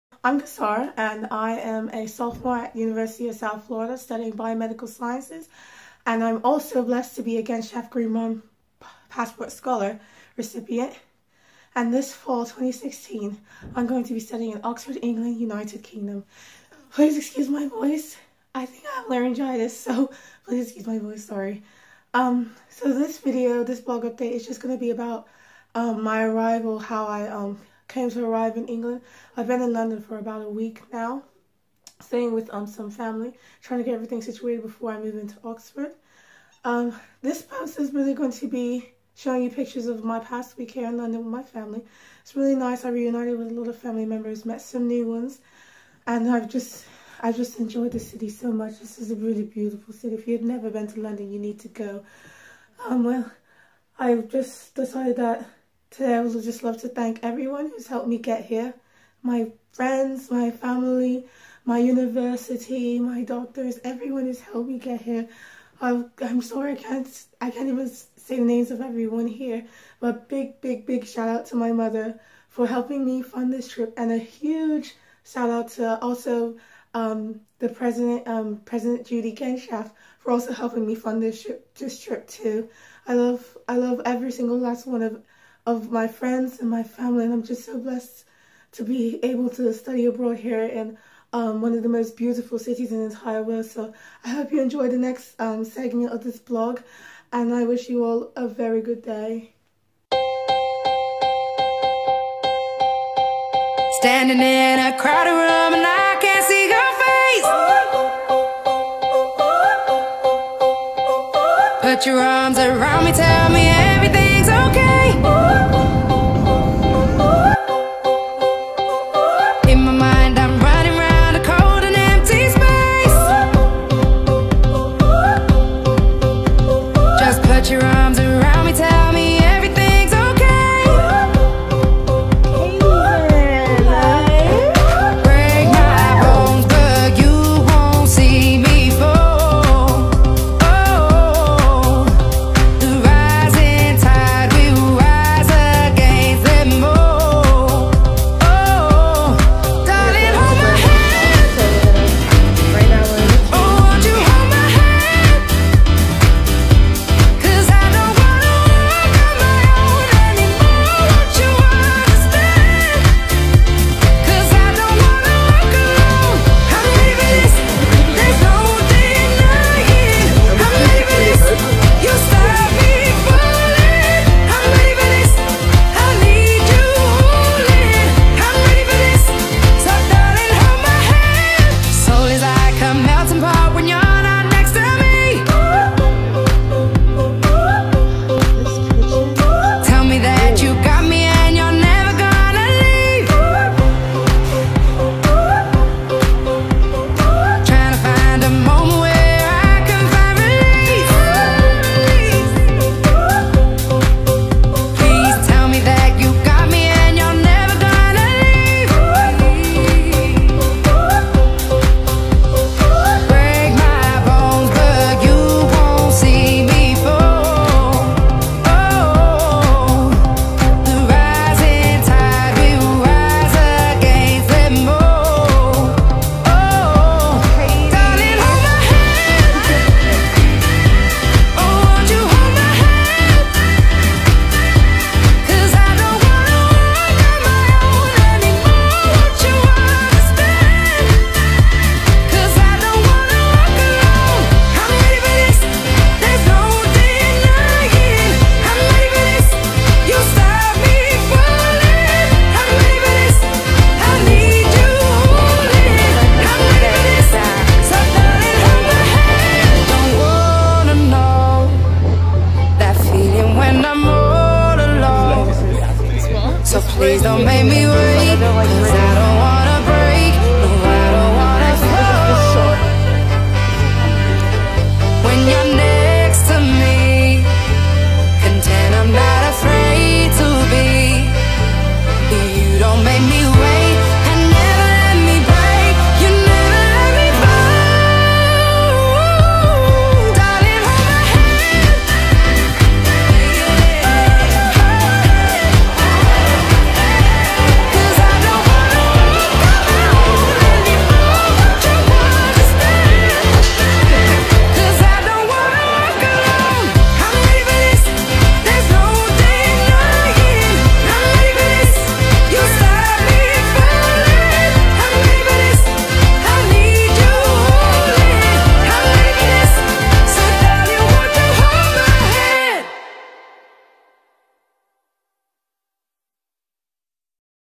First Stop: London, UK
Hi! My name is Kasara and I am a Genshaft/Greenbaum Passport Scholar! This Fall 2016 I have decided to study abroad in the magnificent city of Oxford, England United Kingdom. There is nothing more exhilarating then studying abroad and I recommend it to literally everyone I meet! I decided to spend a week in London with my family before I came to Oxford in order to get myself situated. It has been quite an experience; London is truly the greatest city in the world. I also am very grateful to everyone who has helped me get here- my very generous family and university who has helped me fund this trip. I made this short video detailing my activities before I moved to my new university for this semester, Oxford Brookes University!